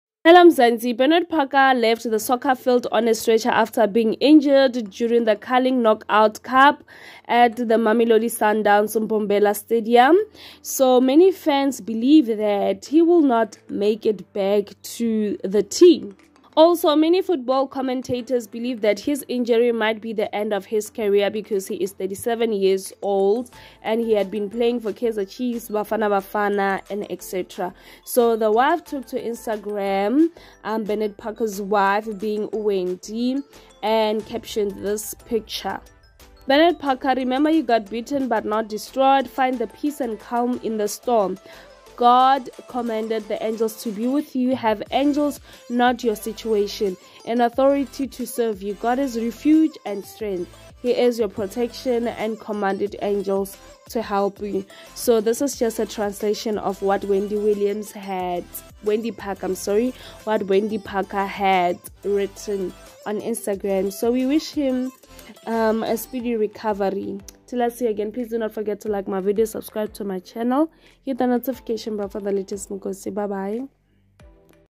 This might be the end of Bernard Parker Career
Bernard Parker (born 16 March 1986) is a South African professional association footballer who plays as a midfielder or forward for Premier Soccer League club TS Galaxy.
It is unclear whether Bernard Parker will be able to continue his career after sustaining what looked like a career-ending injury. The man nicknamed Die Hond was on the receiving end of a crunching tackle from Bongani Zungu during Wednesday's clash between TS Galaxy and Mamelodi Sundowns.2